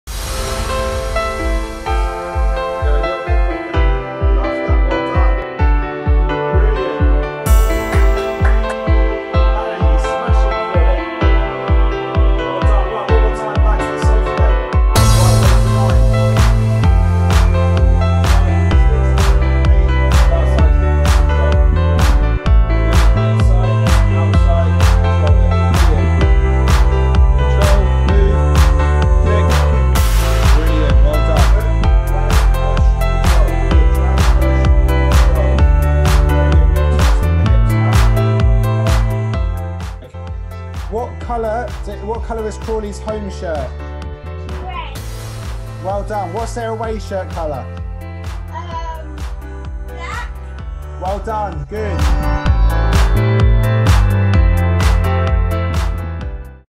ACB121 ONLINE SESSION WITH BRAD | Train At Home With ACB Football Coaching
Another of ACB121 most dedicated footballers Brad kicked off his development with his first online session of 2021.

*ALSO LOVING THE ACB KIT ON DISPLAY FROM BRAD*

Brad was seen striking powerful shots throughout 2020, but has targeted developing his close ball mastery skills into his development plan for this year ahead.

Interested in training with ACB at home? for more info on our availability and how to book with us!